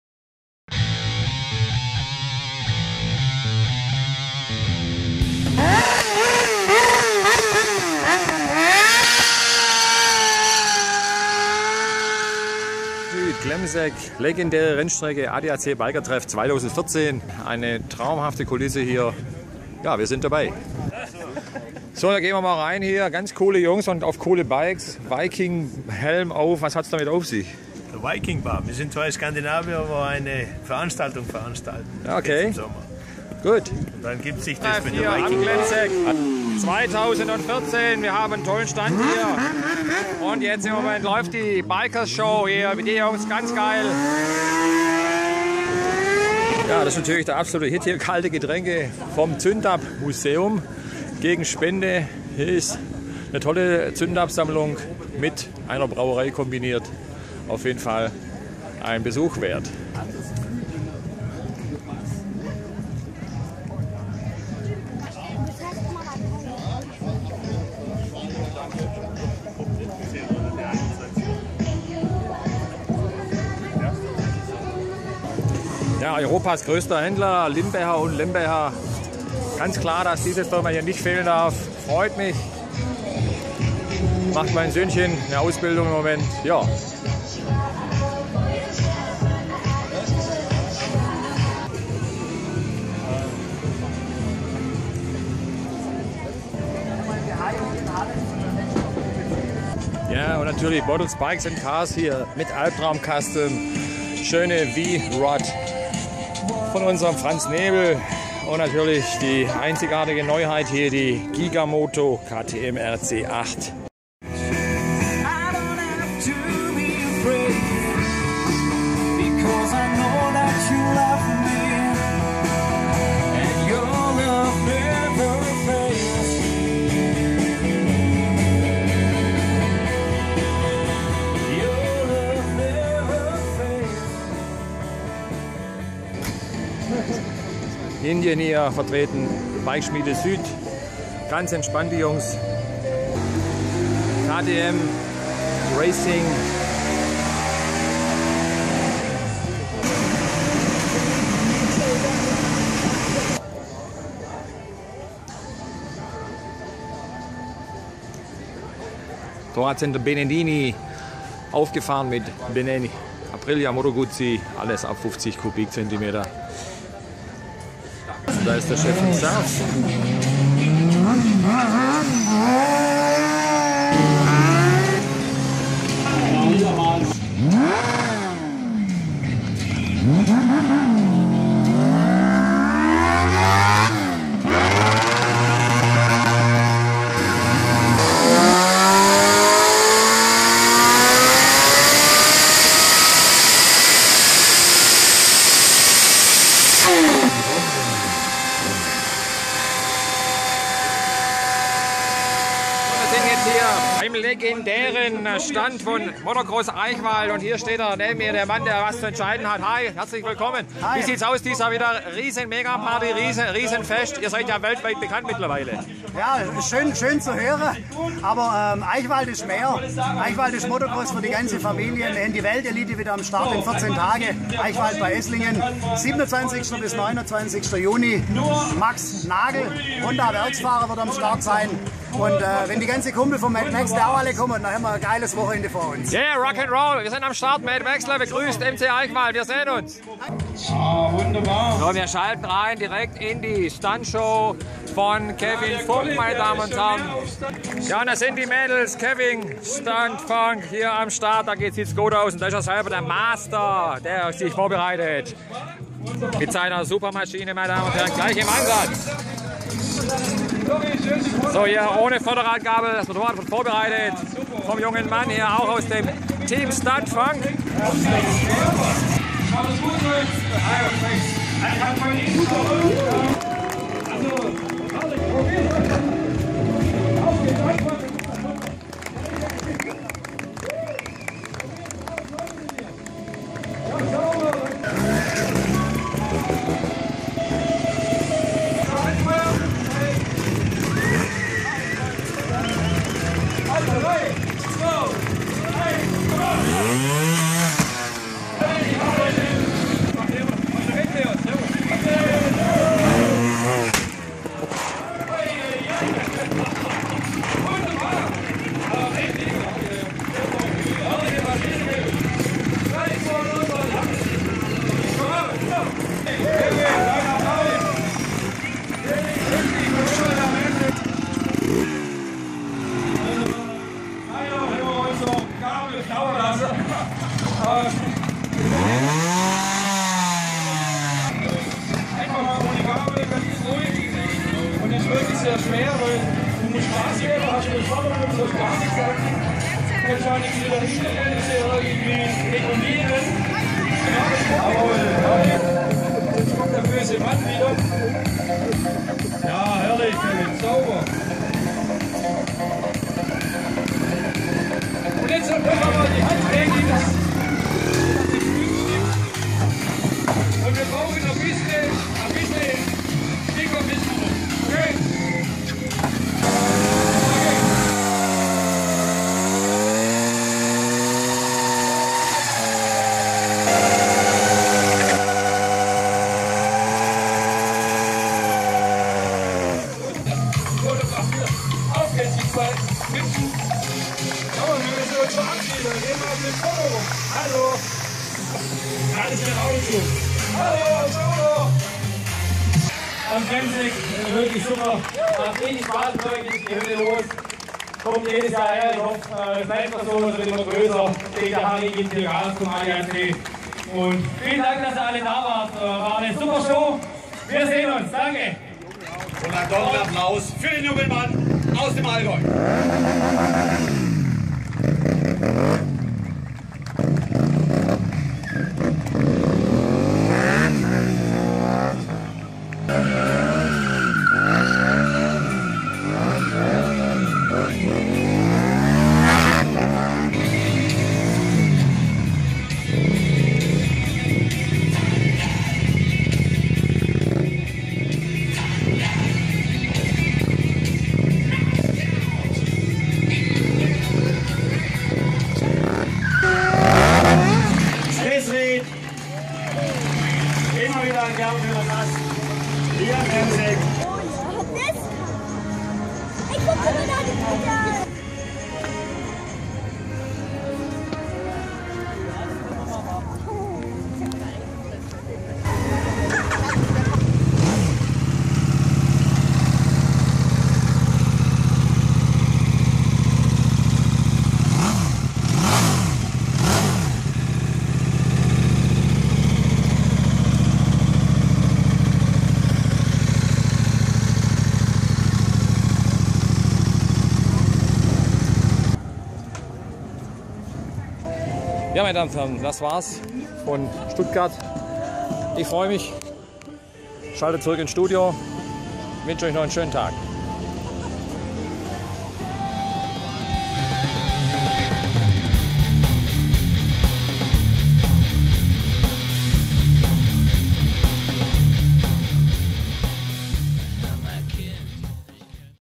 Glemseck Bikertreff 2014 - Boddls Bikes & Cars berichtet in Kurzfassung
Text Quelle: Veranstalterwebseite:

FUN & ACTION -- den ganzen Tag! Unter diesem Motto präsentiert sich der ADAC Bikertreff am Sonntag, den 15. Juni 2014 auf der Verkehrsübungsanlage am Solitude-Ring in Leonberg. Wenn um 9.30 Uhr der Startschuss mit einem Biker-Gottesdienst fällt, können sich die Besucher wieder auf ein erstklassiges Showprogramm mit renommierten Stuntfahrern, zahlreiche Aussteller und einen Quad-Parcours für die kleinsten Motorradfans freuen.

Neben dem Talk mit Freunden und Bekannten dreht sich auch in diesem Jahr wieder alles rund um das Thema Verkehrssicherheit. Die Biker können einen Seh- und Reaktionstest durchführen oder ihre Maschine mal richtig unter die Lupe nehmen lassen. Tipps und Infos hält das Team des ADAC Württemberg für die Besucher bereit: Hier gibt es beispielsweise hilfreiche Tipps zum Kauf eines Motorrads und Infos rund um die Leistungen des ADAC.  Ob TourSet, Fragen zum Motorrad-Führerschein, Ergebnisse der Helmtests, geltende Verordnungen oder Routenvorschläge -- das Servicemobil ist Anlaufstelle Nr. 1!

Alle, die sich über die neuesten Modelle informieren wollen, finden auf der Eventfläche genügend Gelegenheit dazu. Zahlreiche Aussteller präsentieren die aktuellsten Modelle inklusive professioneller Beratung direkt vom Fachmann. Zubehör, Technik, Tuning, Bekleidung und Accessoires runden das Ausstellerangebot ab und lassen keine Wünsche offen.

Auch für die Kleinsten ist gesorgt: Auf die kleinen Motorradfreunde wartet ein Mitmach-Parcours mit Kinder-Quads. So können sich die Kleinen schon einmal auf das Bikerleben einstimmen.

Ein Event für die ganze Familie.

PROGRAMM

9.30 Uhr - 10.15 Uhr

Biker-Gottesdienst

10.15 Uhr
Eröffnung ARENA

10.30 Uhr - 11.00 Uhr SHT Motorradshow
Silke Schober & Jörg Höfle

11.15 Uhr - 11.45 Uhr Quad Driftshow
Chris Rid und Quad Company

12.00 Uhr - 12.30 Uhr Trial-Stuntshow
Kevin Funk

12.45 Uhr - 13.15 Uhr Motorrad Stuntshow
Dominik Csauth

13.25 Uhr - 13.40 Uhr

Interviews

13.45 Uhr - 14.15 Uhr SHT Motorradshow

14.30 Uhr - 15.00 Uhr Quad Driftshow

15.15 Uhr - 15.45 Uhr Trial-Stuntshow
Kevin Funk

16.00 Uhr - 16.30 Uhr Motorrad Stuntshow
Dominik Csauth